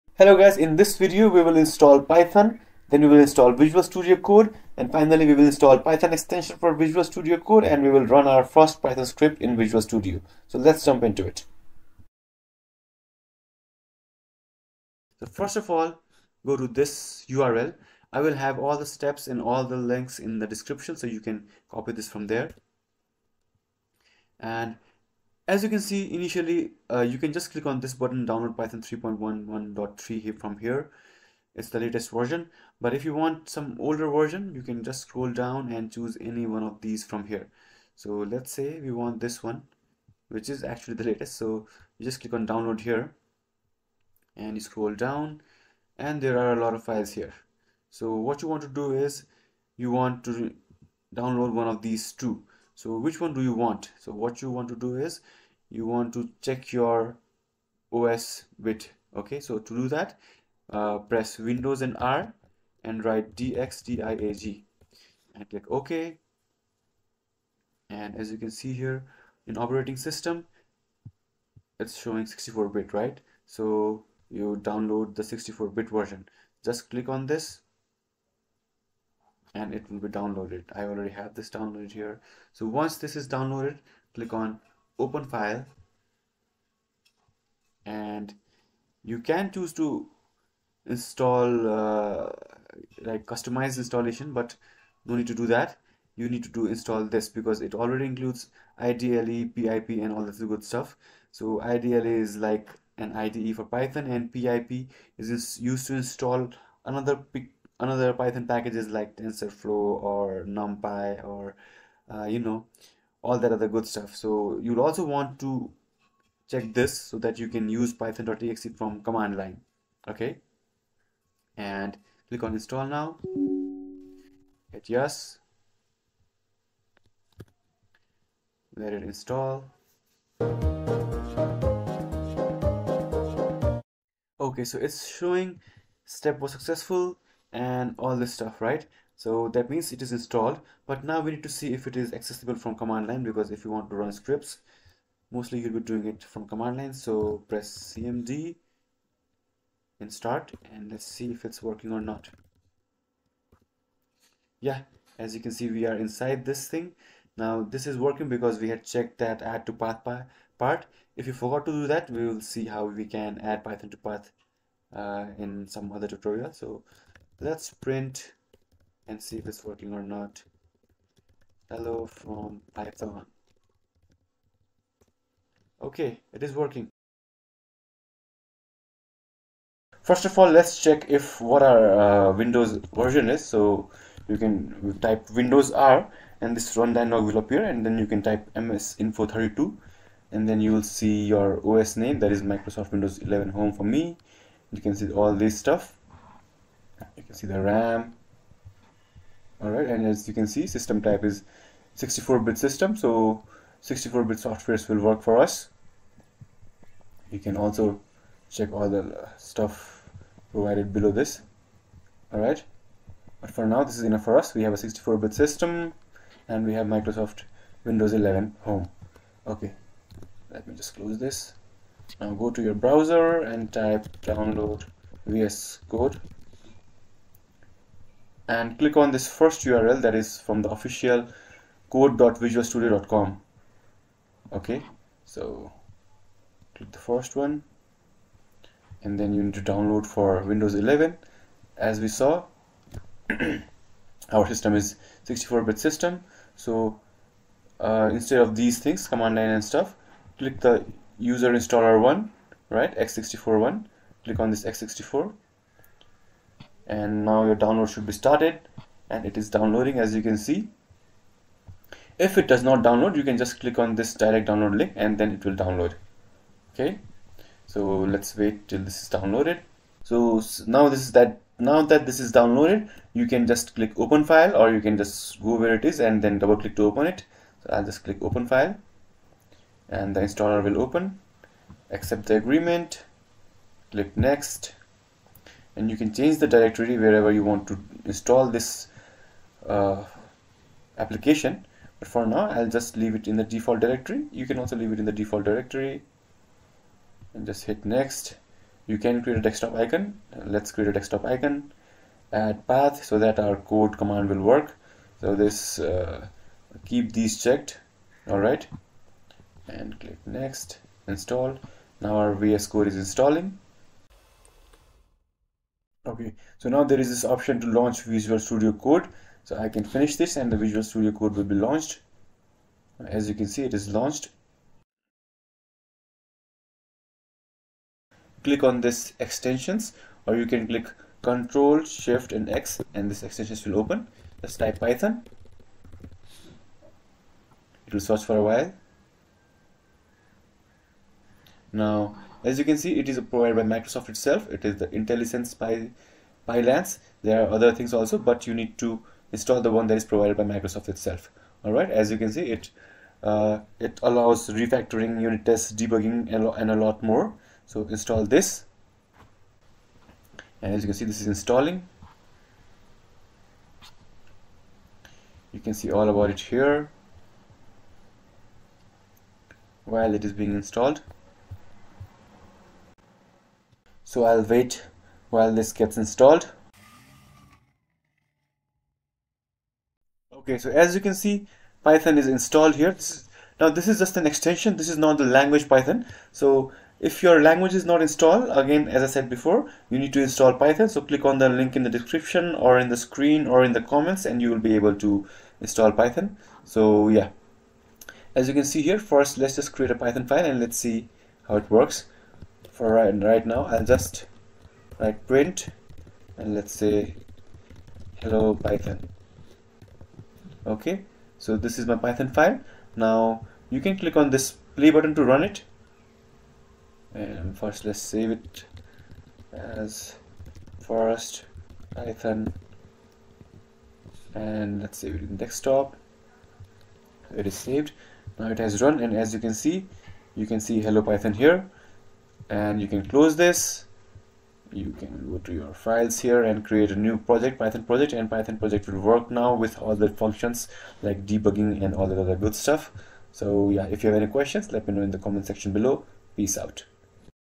How to Install and Run Python and Visual Studio Code (2023)
In this tutorial on how to install and run Python and Visual Studio Code in 2023, I will show you how to get started with Python programming on Visual Studio step-by-step . I will take you  through the process of installing Python, Visual Studio Code, and the Python extension in Visual Studio Code, ensuring you have everything you need to embark on your coding journey.

Python is a versatile and widely-used programming language known for its simplicity and readability. Visual Studio Code, on the other hand, is a popular and powerful code editor that offers a wide range of features and extensions for efficient coding. By combining Python and Visual Studio Code, you'll have a robust development setup that will streamline your coding workflow.

By the end of this video, you'll have a fully functional Python development environment set up with Python, Visual Studio Code, and the Python extension. Whether you're working on web development, data analysis, or machine learning projects, this tutorial will equip you with the necessary tools to write clean and efficient Python code.